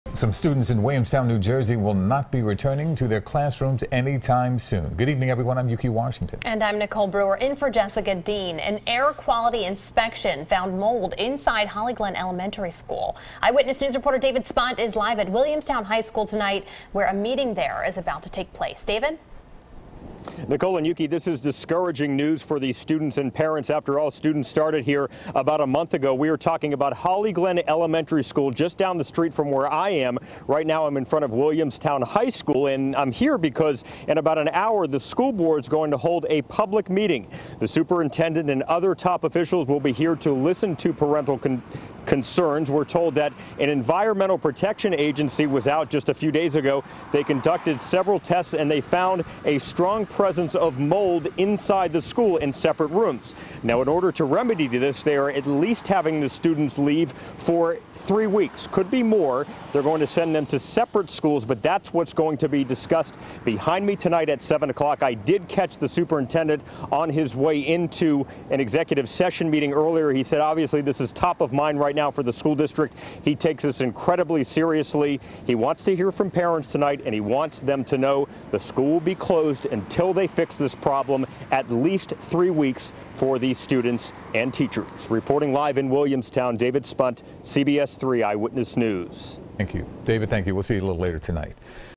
Mold Found In New Jersey Elementary School
David Spunt reports from Williamstown, NJ.